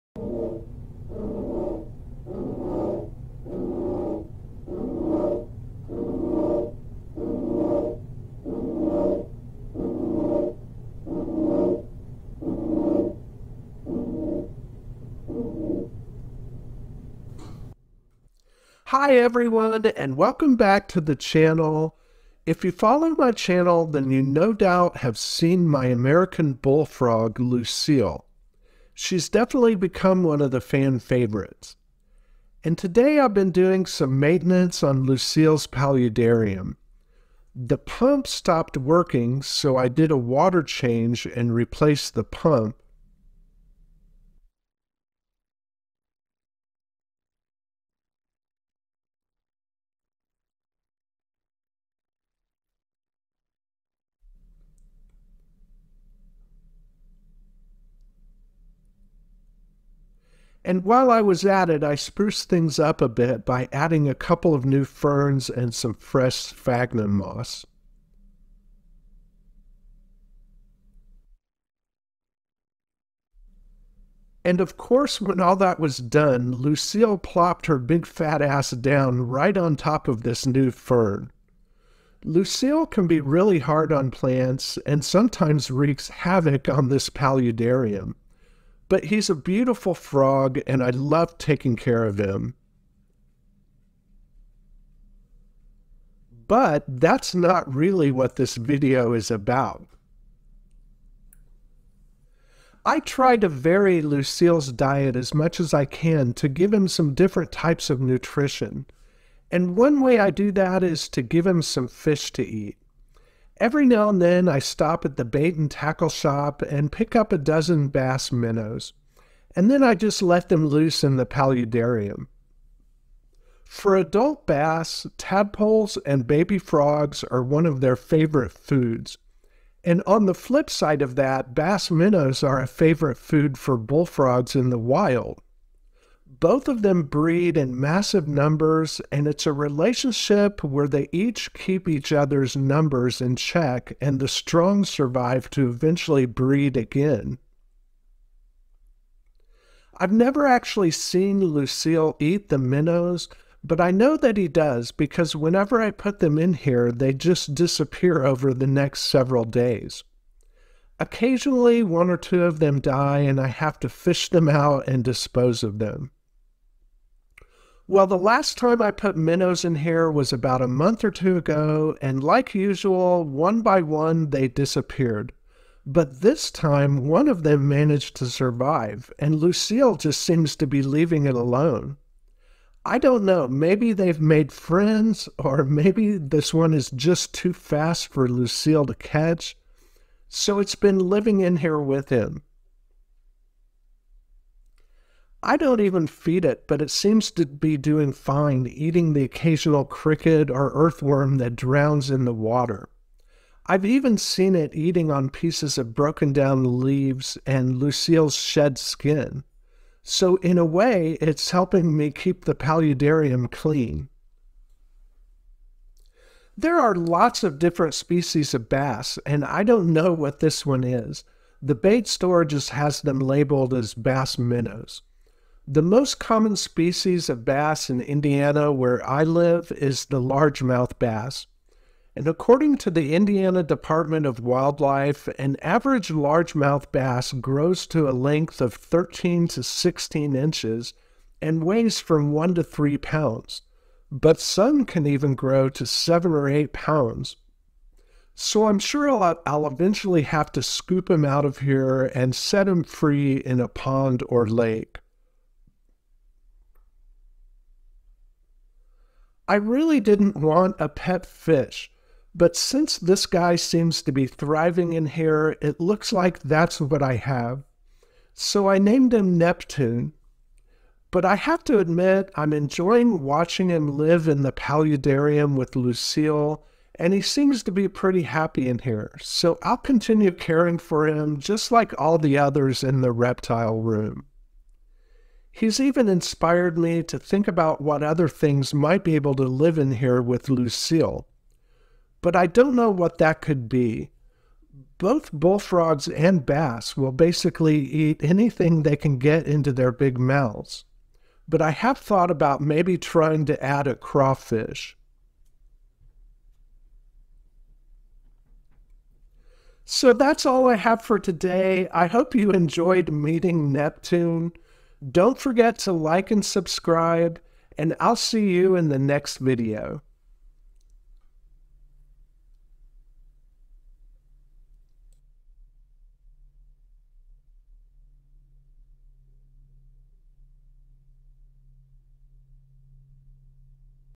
I Didn't Want a Pet Fish!
I didn't want a pet fish, but somehow that's what I have. I sometimes feed my pet bullfrog, Lucille, bass minnows and she usually devours them. But this time one of them survived and Lucille's been leaving it alone. It's been thriving in the paludarium for a couple of months now, so I'm just taking care of it like all of the other animals in the reptile room. I named him Neptune. I didn't want a pet fish, but I've actually been enjoying watching taking care of him in the paludarium with Lucille. I hope you enjoy meeting Neptune.